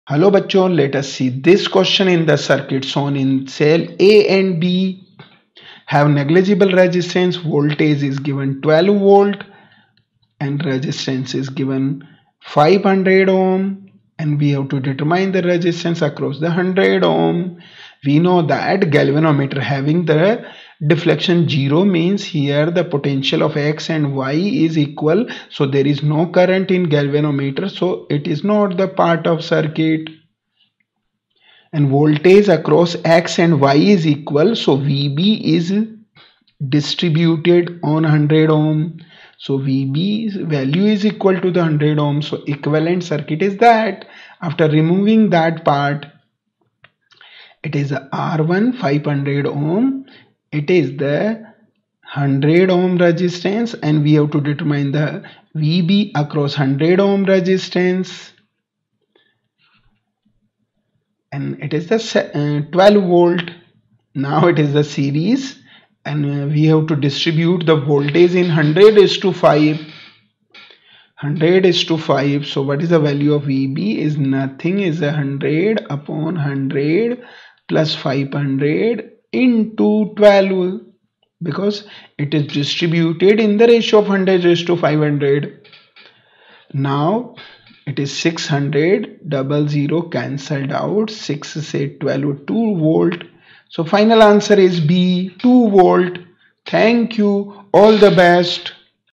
In the circuit shown the cells A and B have negligible resistances. For V_A=12 V, R_1=500 Ω and ...
In the circuit shown the cells A and B have negligible resistances. For V_A=12  V, R_1=500 Ω and R=100 Ω the galvanometer (G) shows no deflection. The value of V_B is:
(2012 Pre)
b. 2  V
c. 12  V
d. 6  V